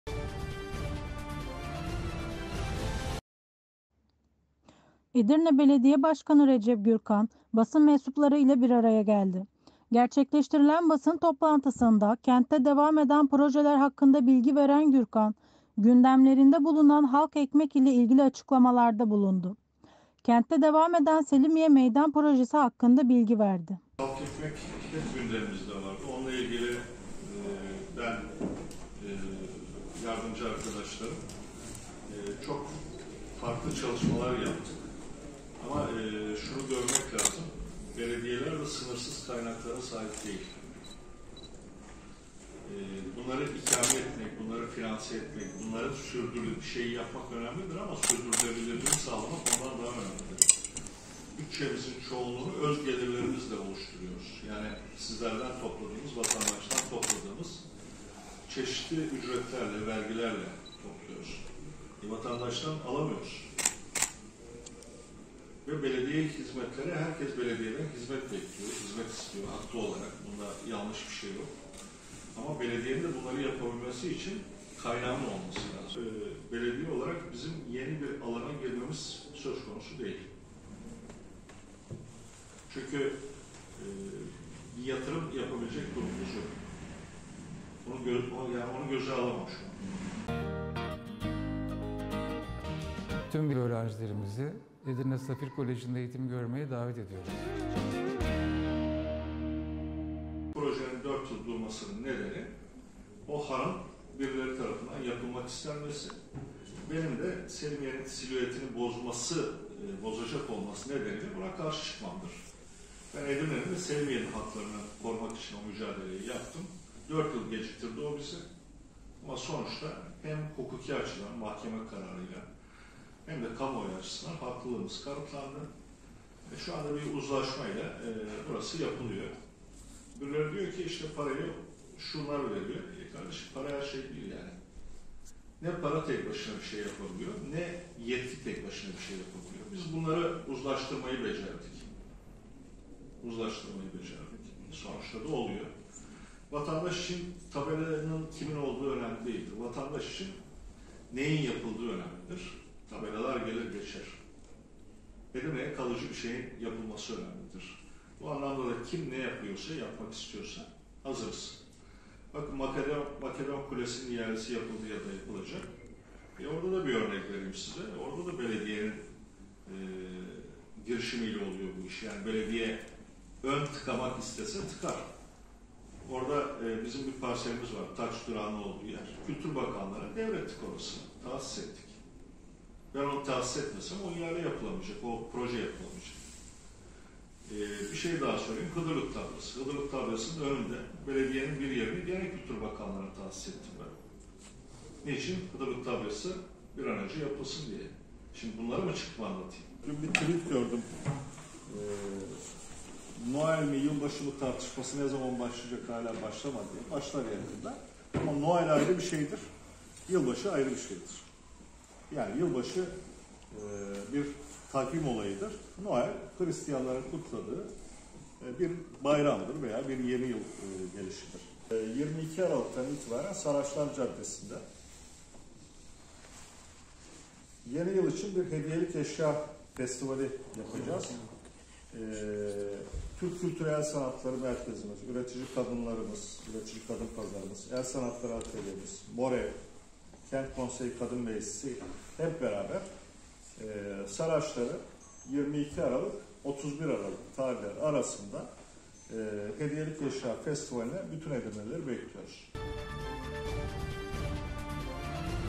Başkan Gürkan’dan halk ekmek açıklaması
Edirne Belediye Başkanı Recep Gürkan, basın mensuplarıyla bir araya geldi. Gerçekleştirilen basın toplantısında kentte devam eden projeler hakkında bilgi veren Gürkan, gündemlerinde bulunan halk ekmek ile ilgili açıklamalarda bulundu kentte devam eden Selimiye Meydan Projesi hakkında bilgi verdi.